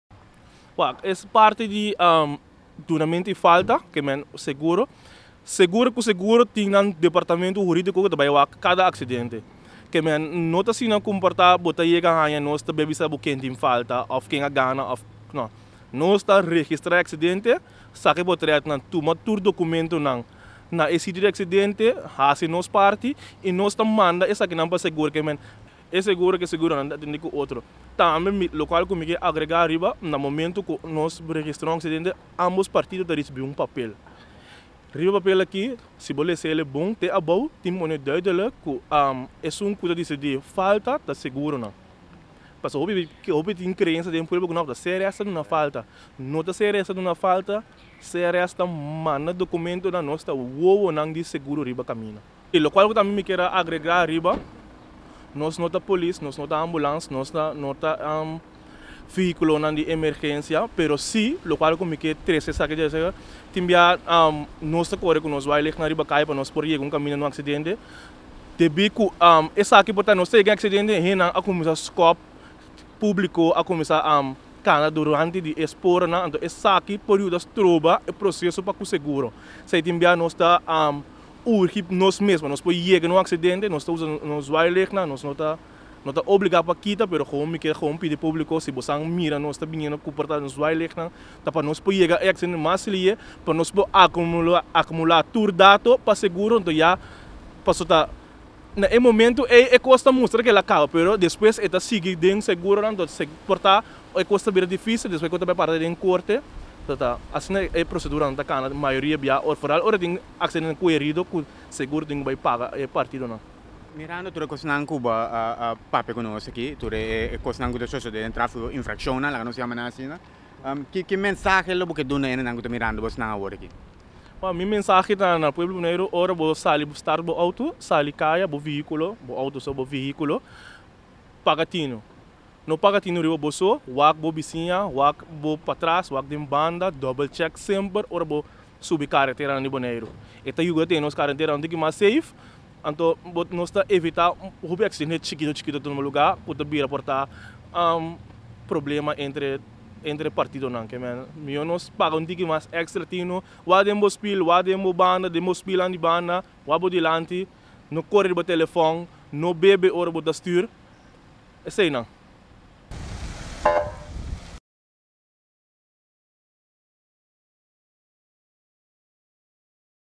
(3) Caribbean Road Service reportahe di trabou i sifranan aksidente na Boneiru.
Hopi bia outomobilistanan ku haña nan mes envolvi den aksidente di trafiko ta kere ku ta C.R.S ta tuma e ultimo desishon pa loke ta trata falta den trafiko, pero sigun sr emerensiana ta seguronan ta hasi e investigashon i saka konklushon.